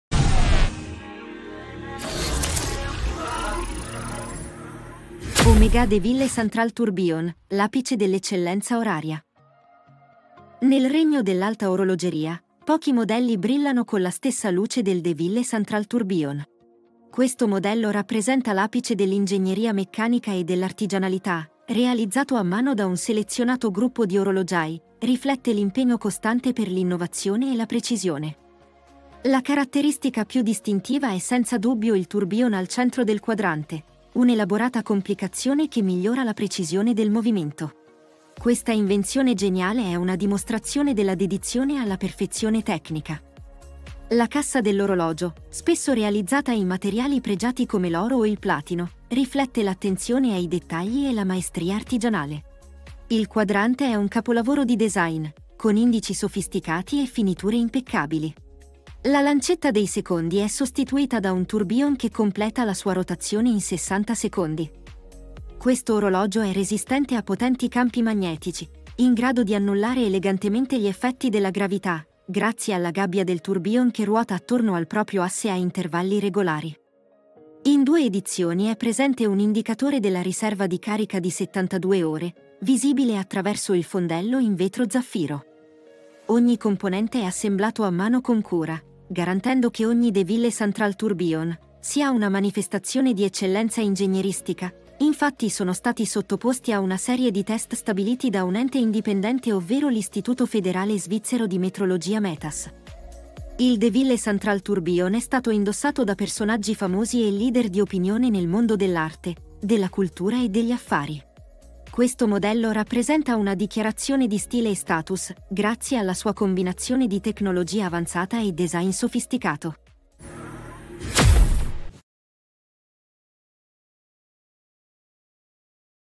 OMEGA DE VILLE CENTRAL TOURBILLON: l’orologio che unisce arte orologiera e innovazione tecnologica
Scopri il De Ville Central Tourbillon, il modello di Omega che combina l’artigianalità, il design sofisticato e la tecnologia all’avanguardia. Un orologio da polso che rappresenta l’apice dell’eccellenza oraria e della perfezione meccanica.